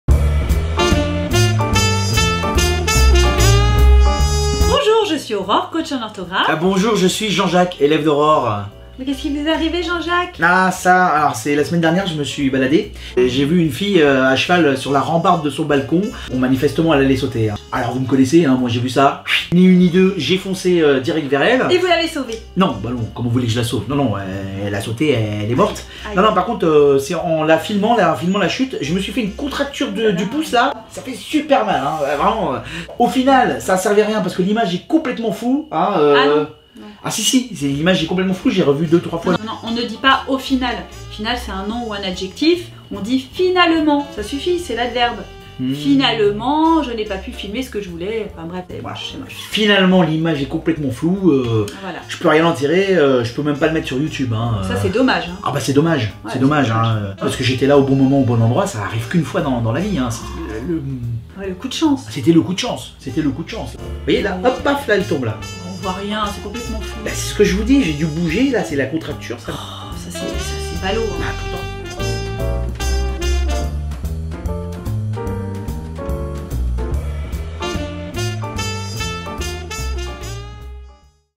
Petite leçon de français : "Au final"
Retrouvez tous les 15 jours une petite leçon de français avec Aurore et Jean-Jacques.